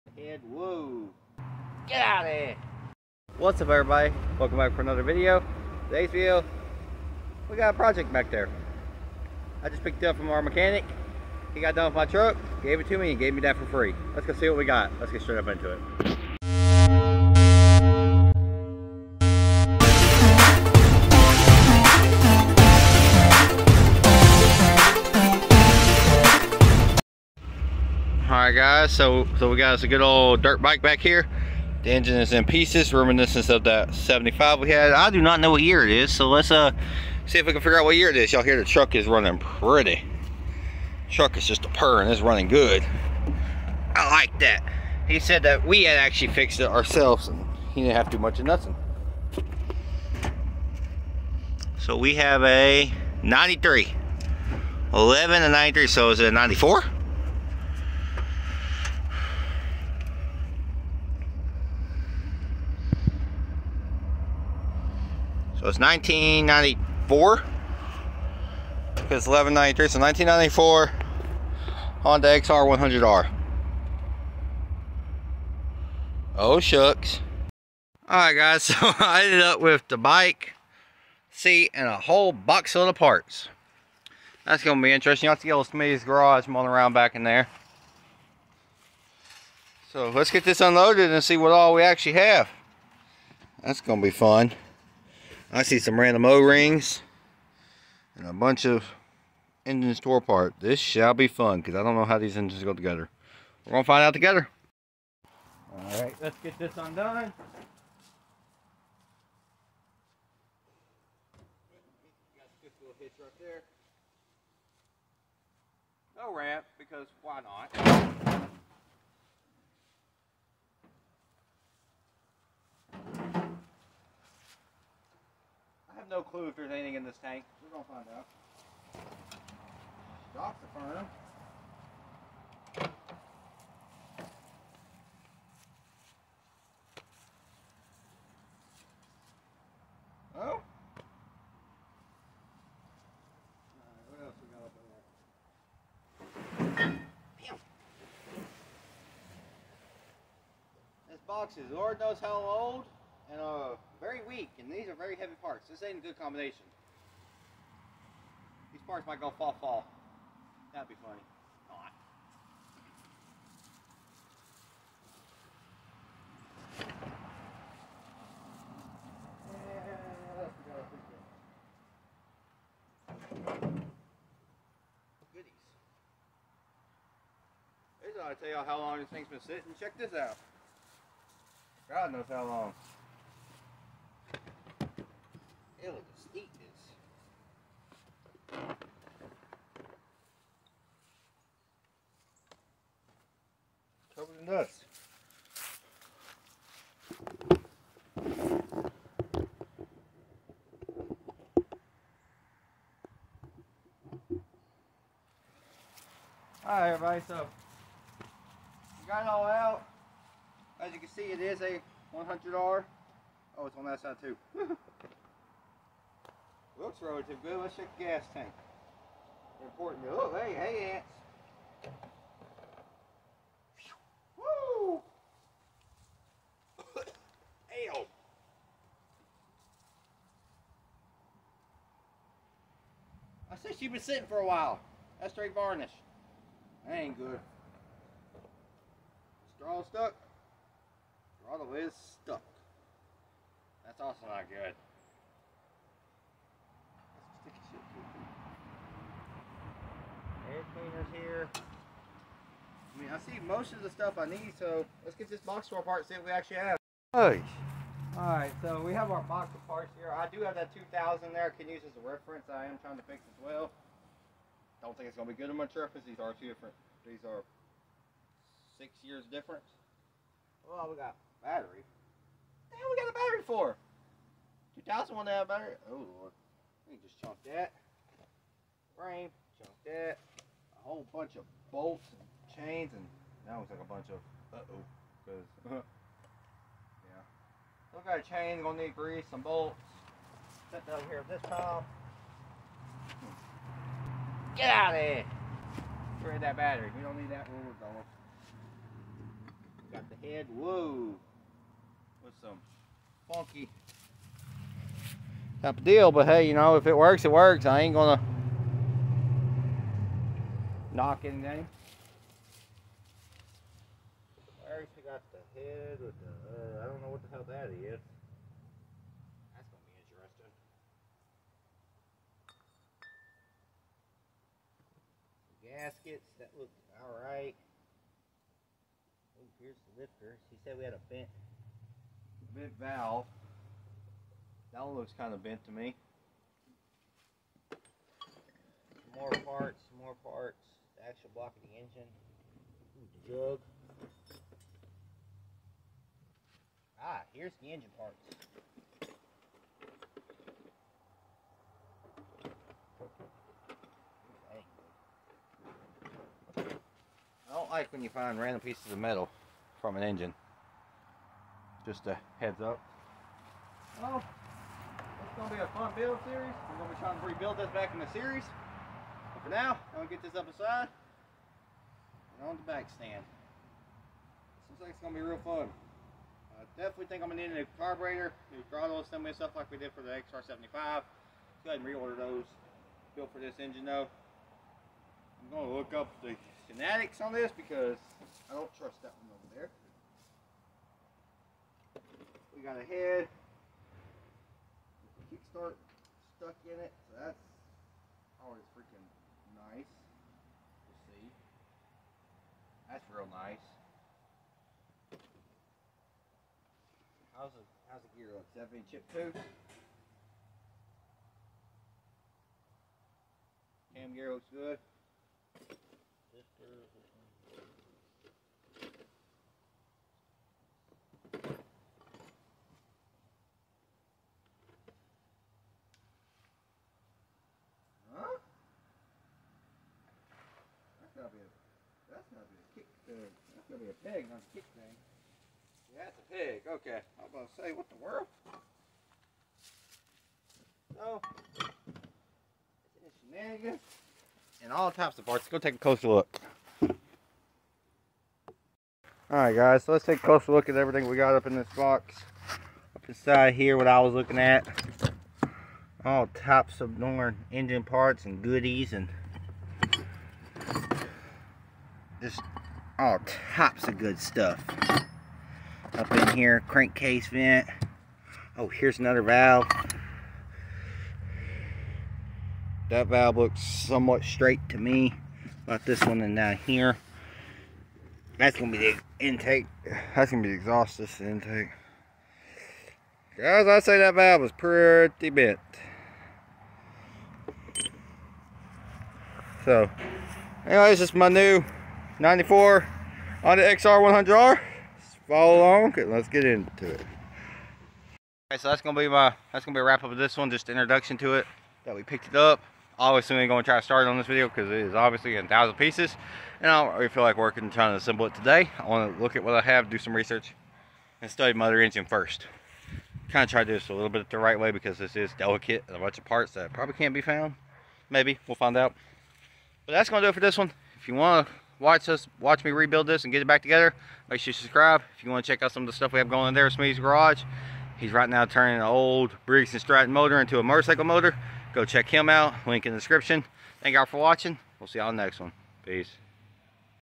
I got a 1994 Honda xr100r for free!!!
In today's video we take a look at the 1994 Honda xr100r I picked up for free and see what all I got with it. make sure to subscribe and hit that like button for this series